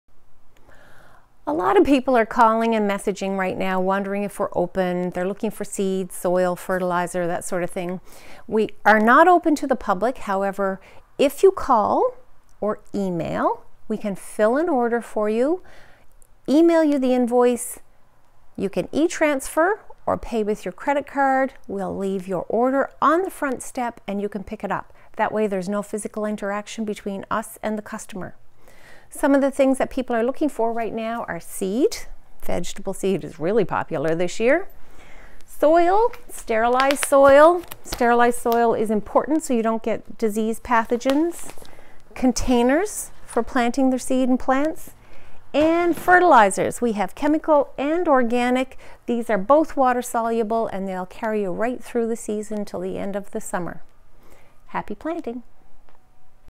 Kim Talks Ordering Due to CoVid-19
Kim chats about how to order in light of social distancing and the closure of browse and shop practices